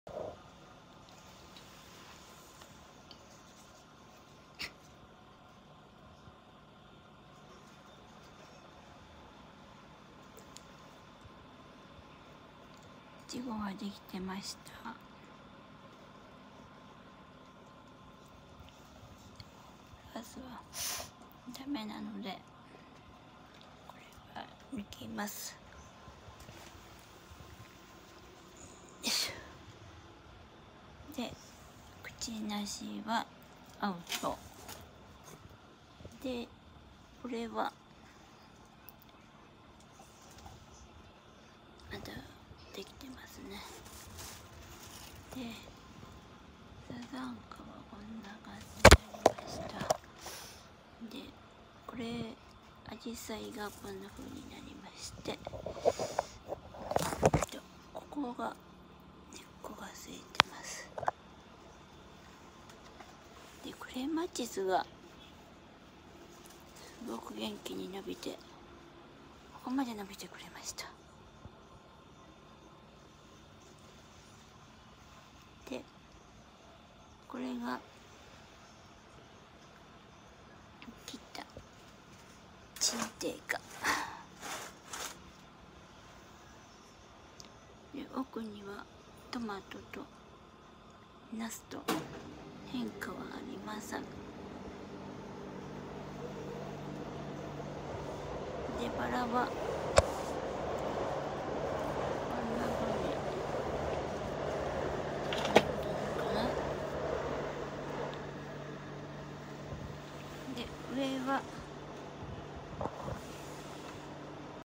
入院後の花の様子①
入院後なので声もガラガラ、ベランダも見事な荒れ放題になってます。
クチナシは綺麗に枯れました。
苺は一つだけ出来てました。
唯一の救いがクレマチスの伸びでした。
入院される方はご参考にして下さい。